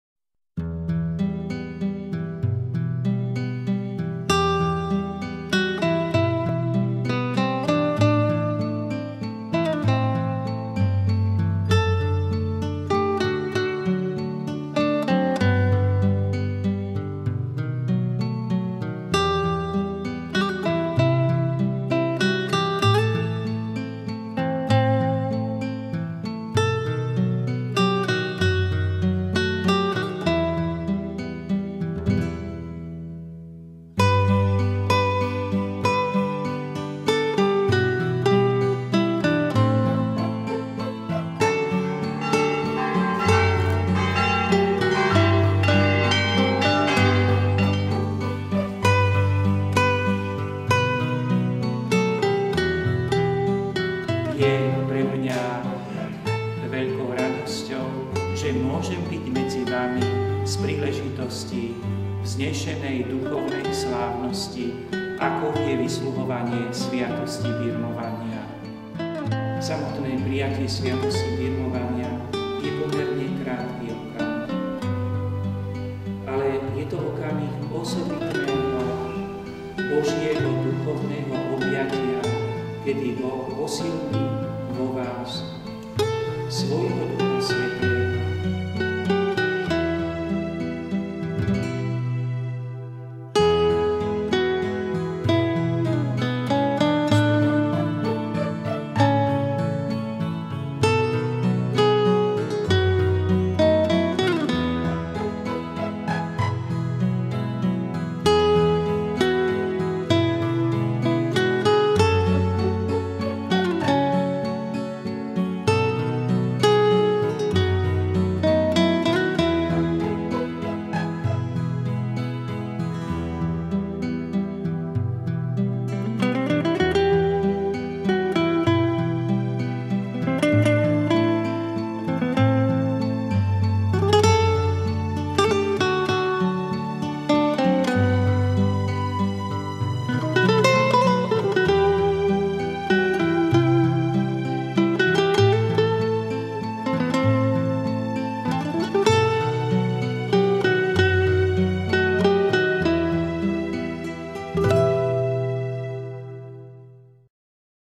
Sviatosť birmovania - Senec 29.10.2011
videoklip z vysluhovania sviatosti birmovania - Kostol svätého Mikuláša v Senci - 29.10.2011 - HMC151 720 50P